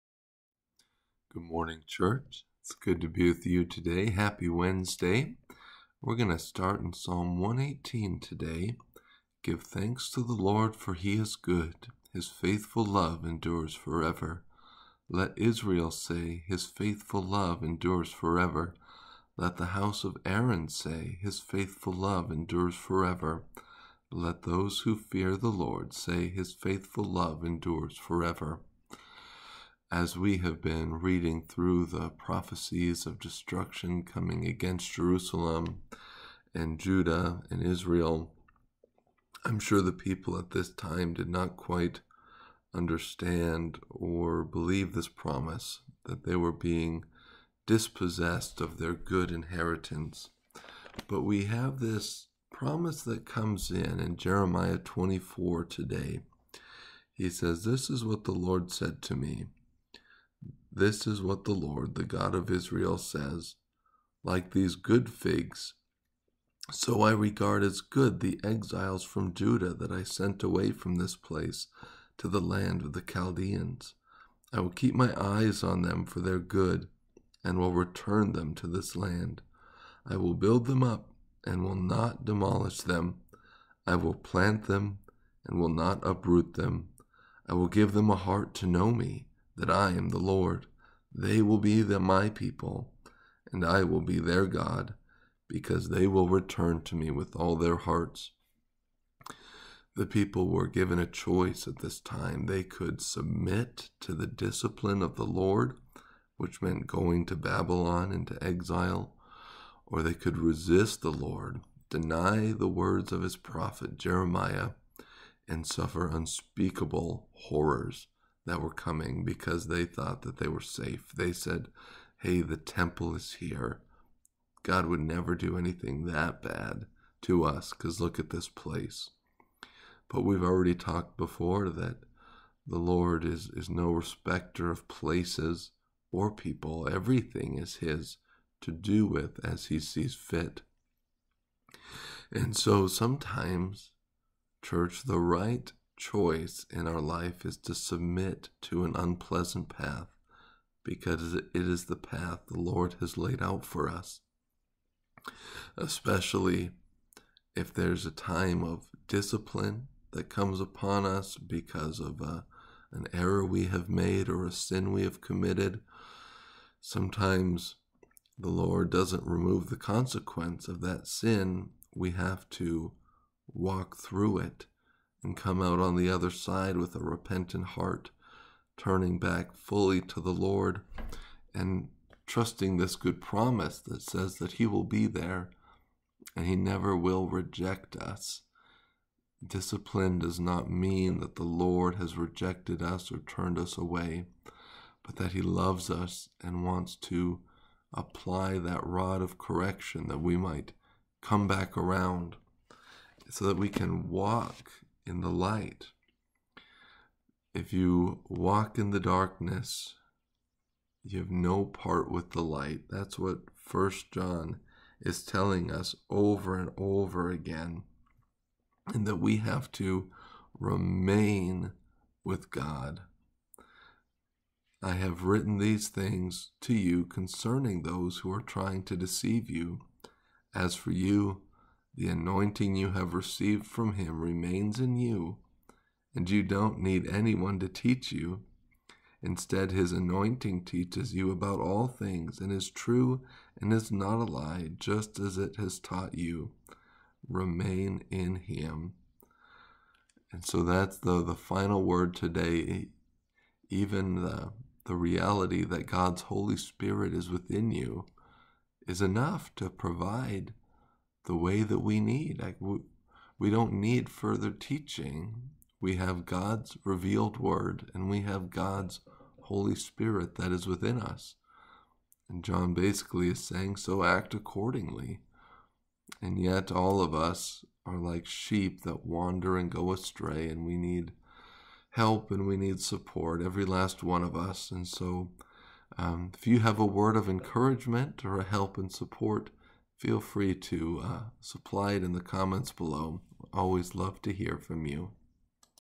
Day 234, Psalm 118
Day 234: Jeremiah 21,24,27; Psalm 118; 1 John 2
There is great joy in knowing our God will never leave us or forsake us, even through the times when God applies correction and discipline to our lives it is only because of his everlasting love. Let us turn to him today and walk in the hope of salvation.